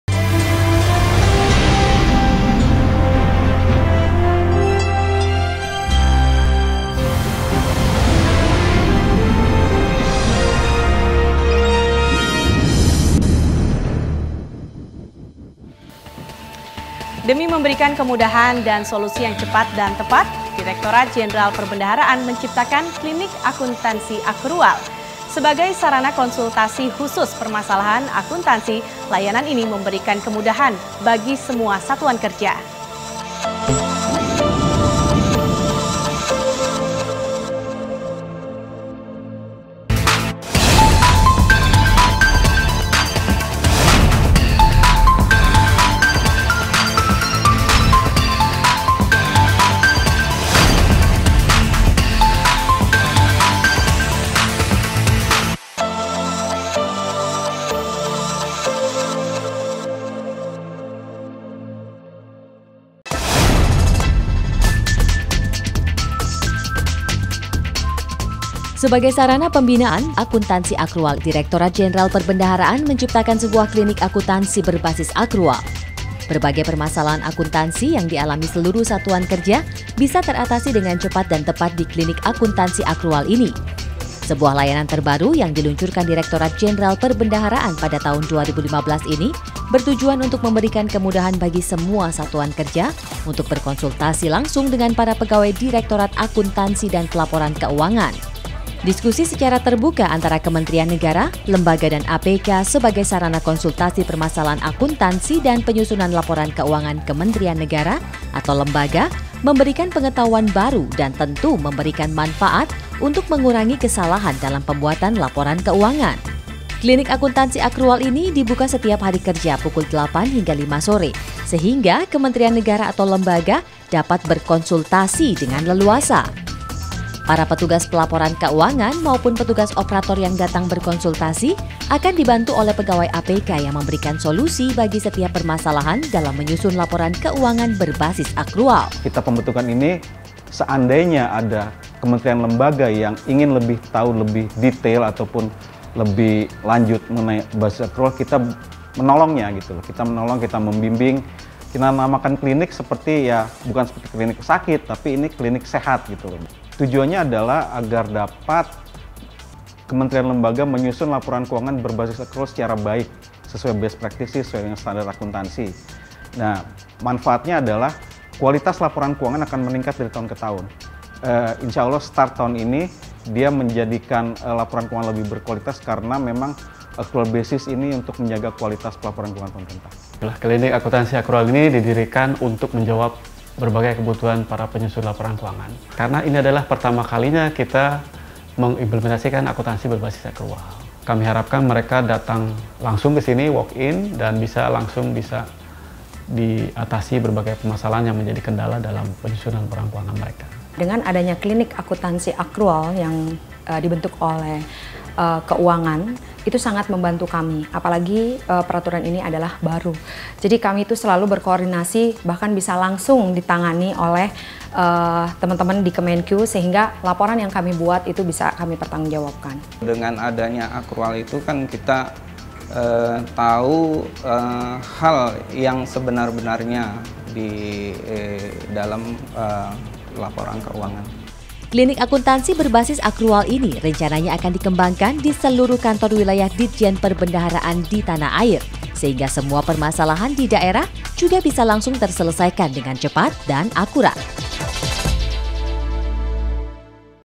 Peluncuran Klinik Akuntansi Akrual
Klinik Akuntansi Akrual merupakan sarana untuk Satuan Kerja berkonsultasi masalah Akuntansi Akrual dengan harapan semua Kementerian mendapatkan predikat WTP. Klinik ini adalah inisiatif Direktorat Akuntansi dan Pelaporan Keuangan Direktorat Jenderal Perbendaharaan